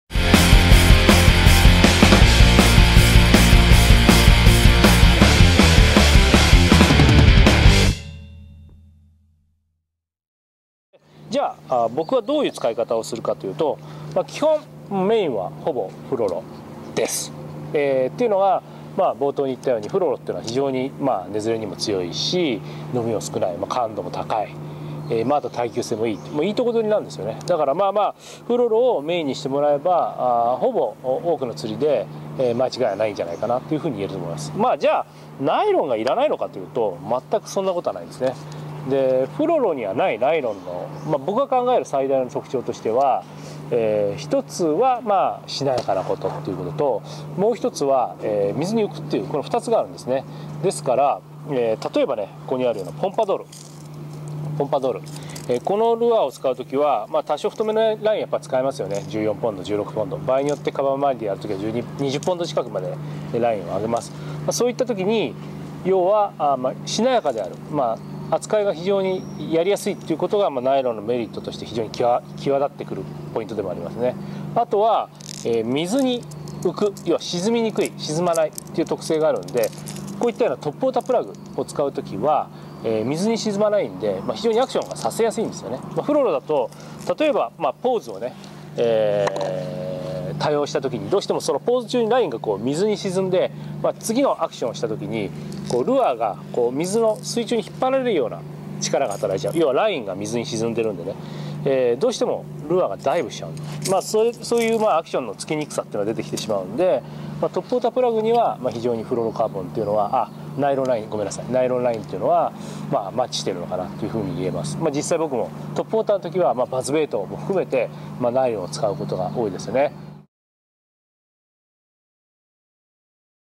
2014.04.11 ラインセレクトの基礎知識Vol.2
メインではフロロカーボンを使用するが、ナイロンにはその特性と有効な使い方があると語る小野プロ。基本に立ち返りながら解説してもらいます。